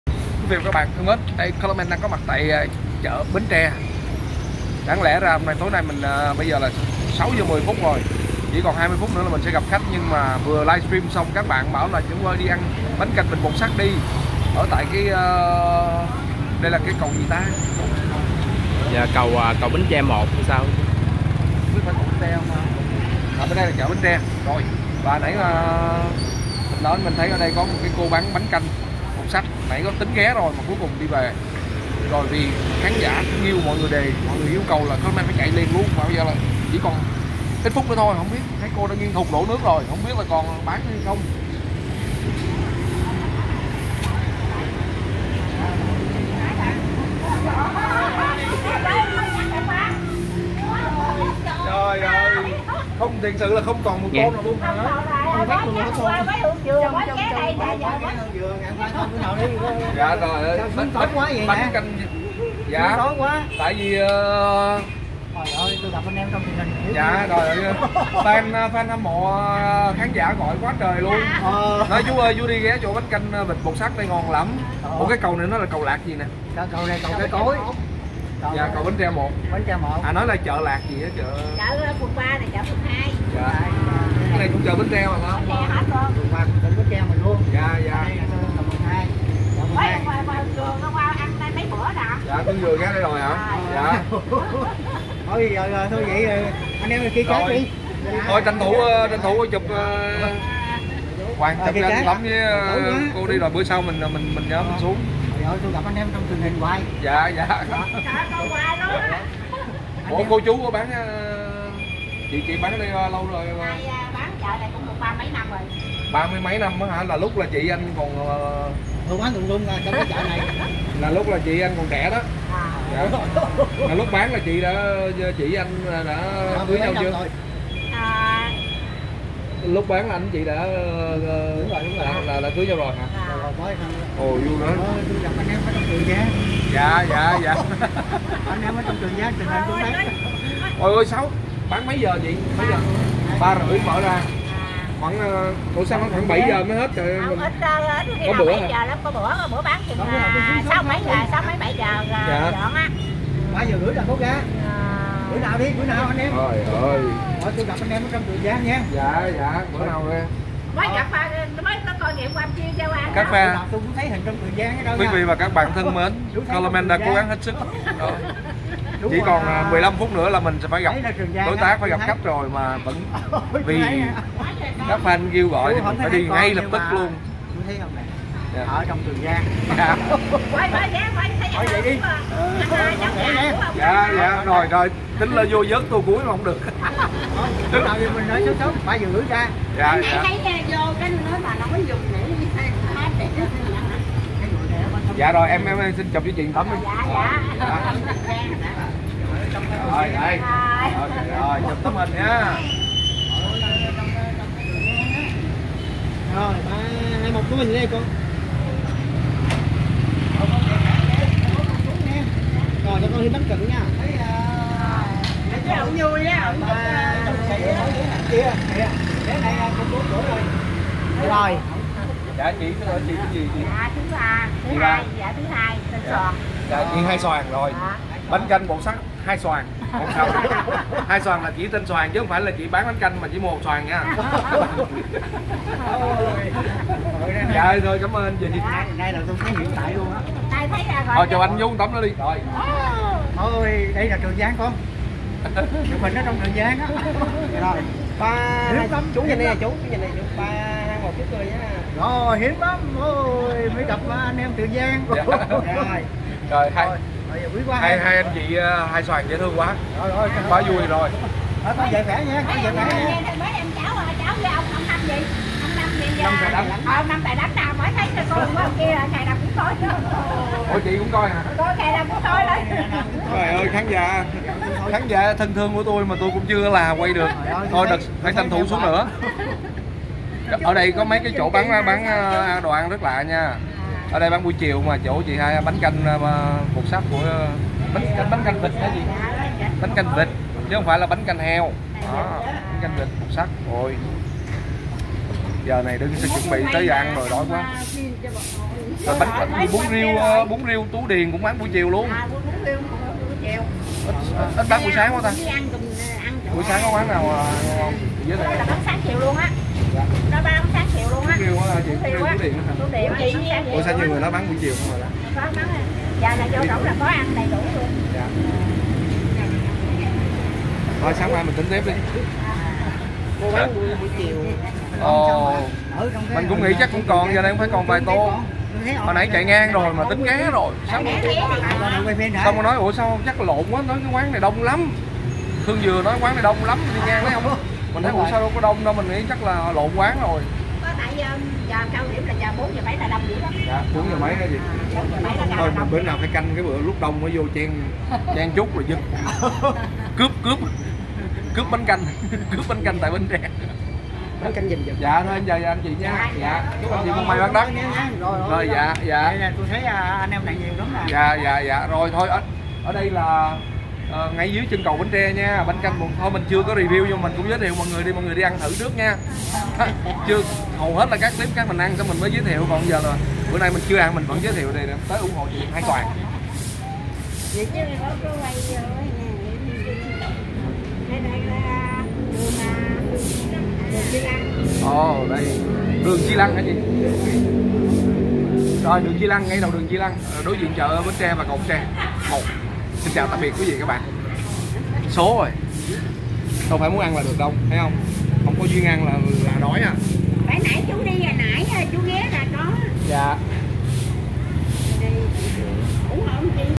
Có Những Niềm Riêng #25: 3 chân 4 cẳng phóng đi review bánh canh vịt bột xắt nhưng...
Nằm ở đầu đường Chi Lăng, chân cầu Bến Tre 1 có 1 quán bánh canh bột xắt vịt của cô Hai Xoàn khá nổi tiếng. Được fan giới thiệu Color Man 3 chân 4 cẳng phóng đi review nhưng cái kết thật đắng lòng.